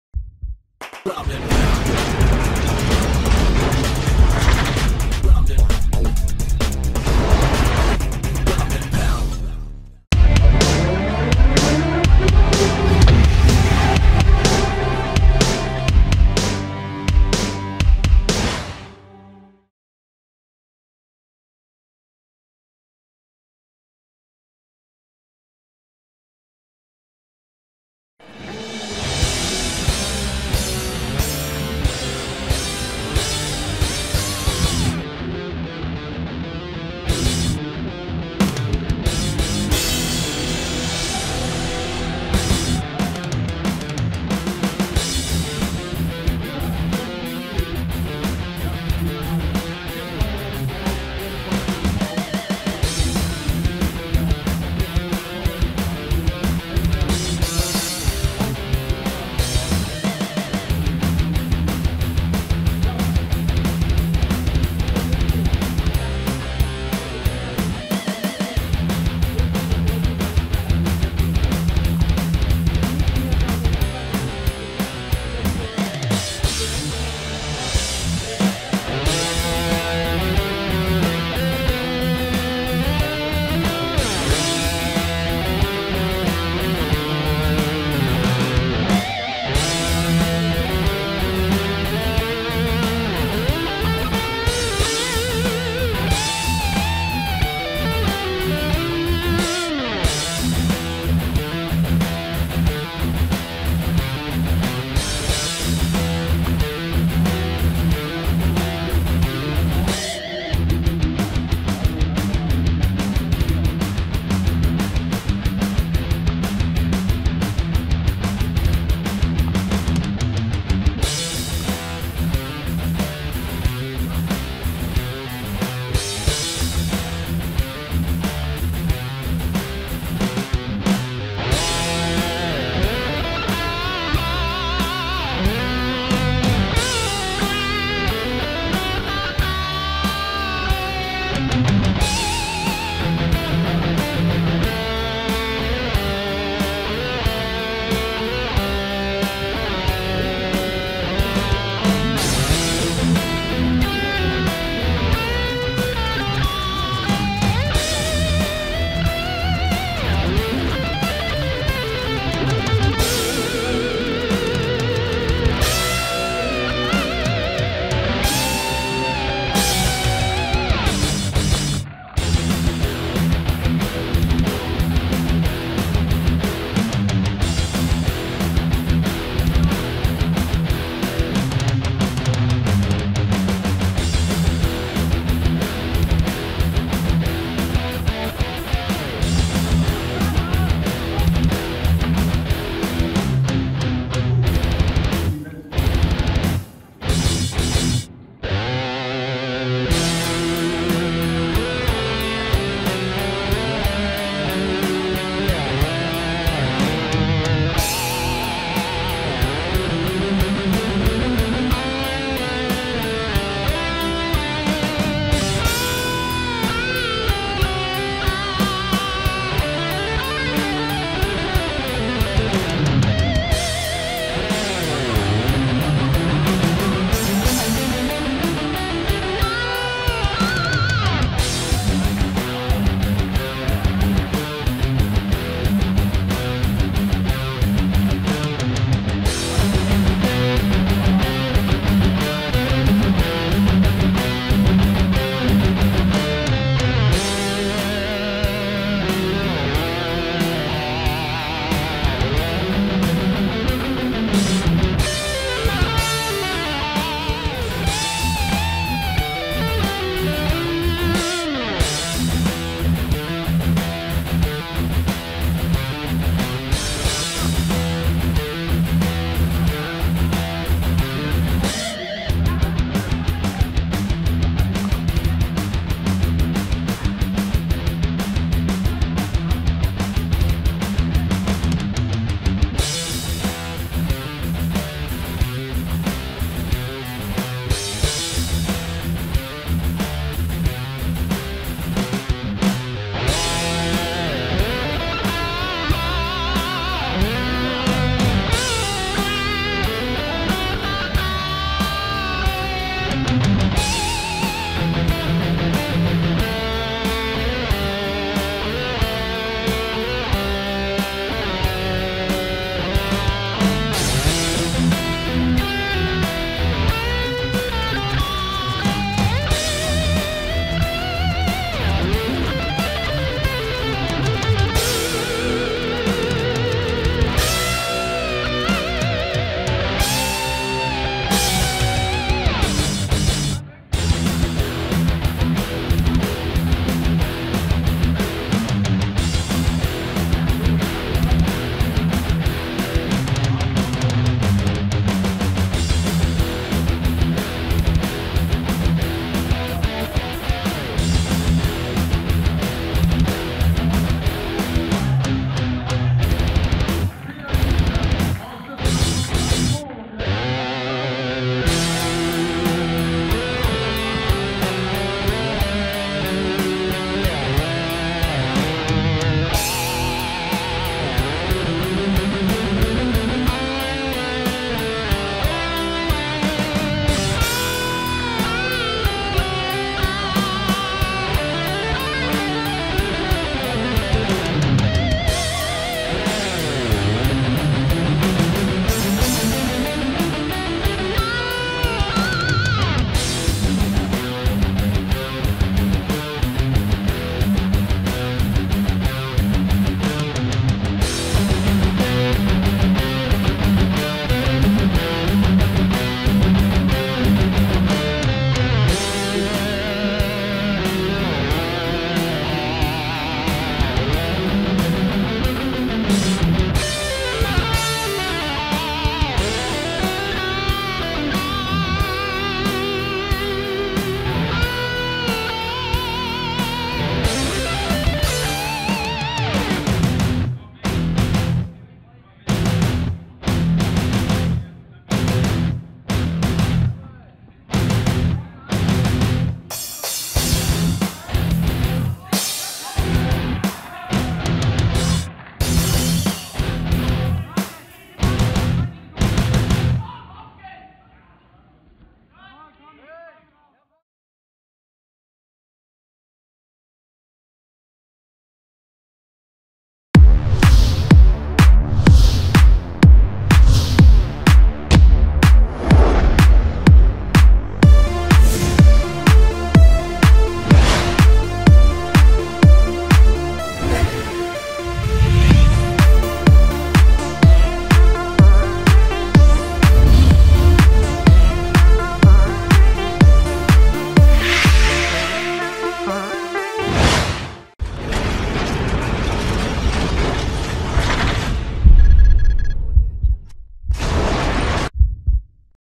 DGL Finale 2012 - Highlights
Impressionen vom Finale der Deutschen Grappling Liga (DGL) vom 17. November.